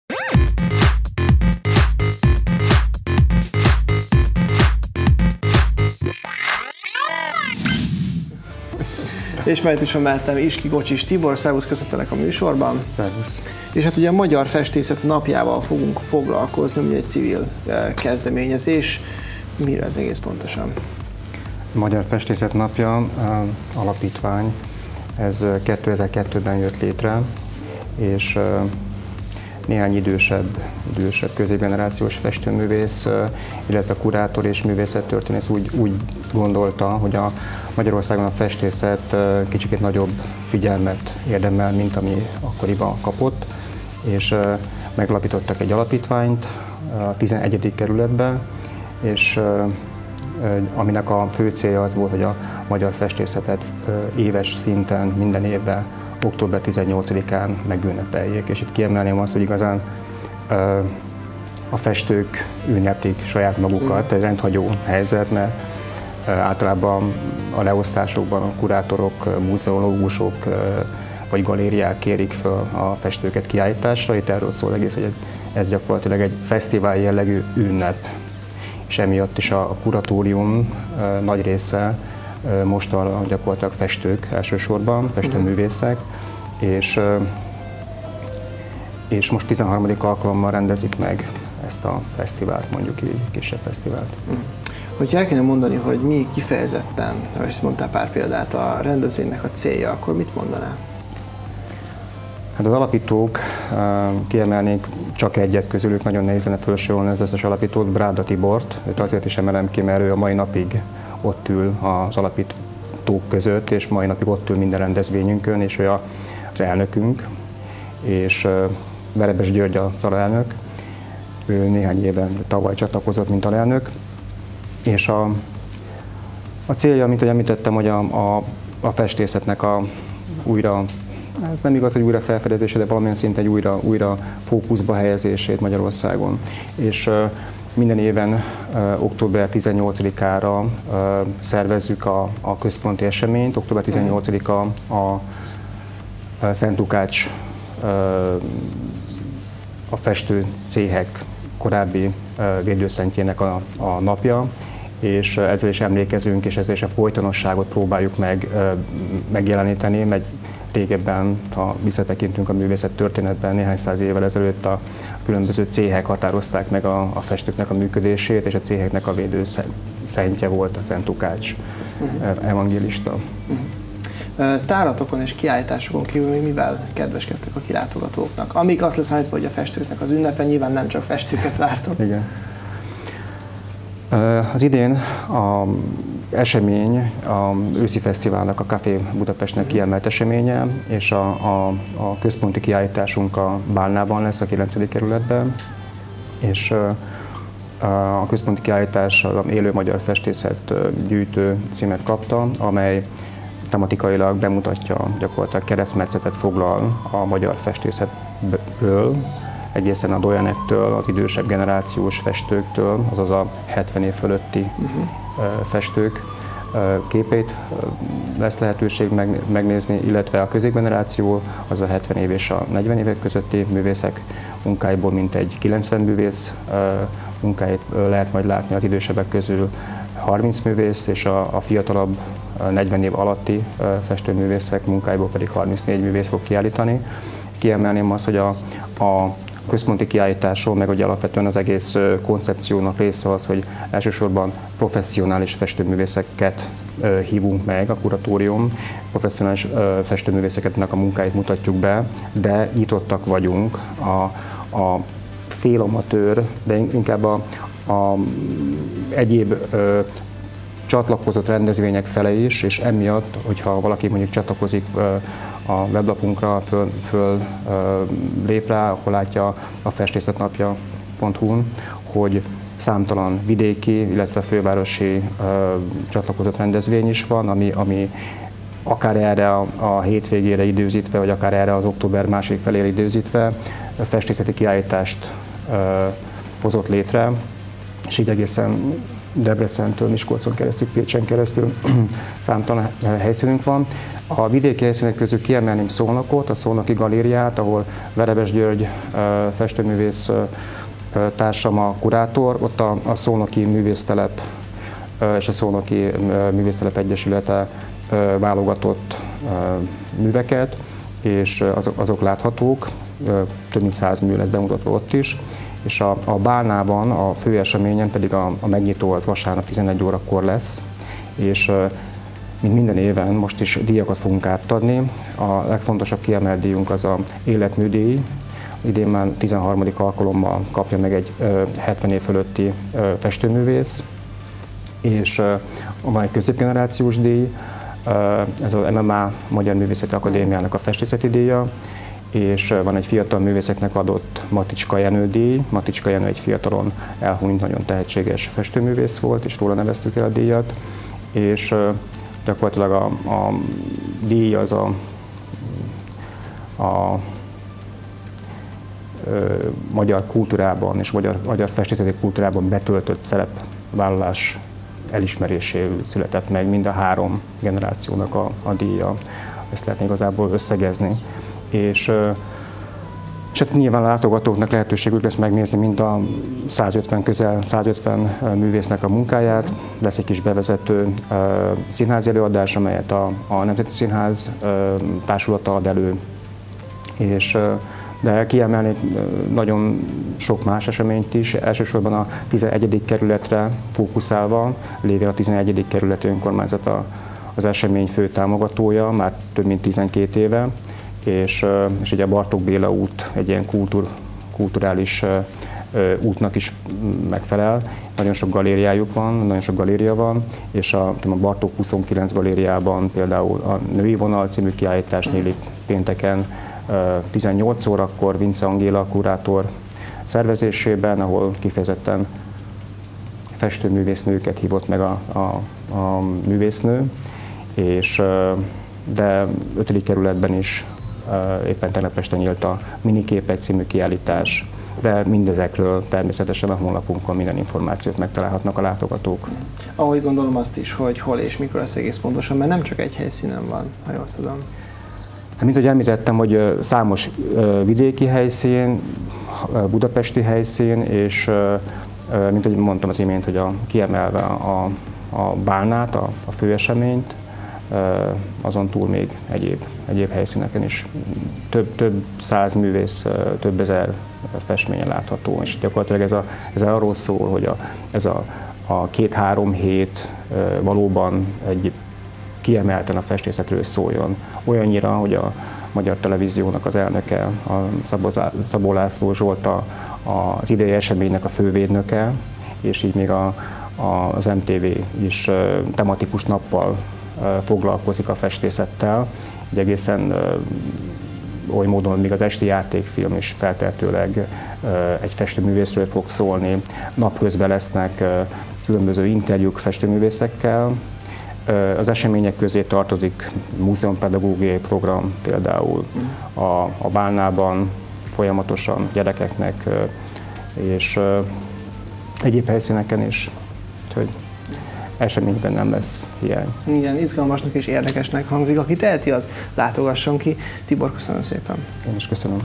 FIX TV | FIX Magazin - Festészet Napja | 2014.10.14.
Vendég: Iski Kocsis Tibor - festőművész, kuratóriumi tag - Magyar Festészet Napja Alapítvány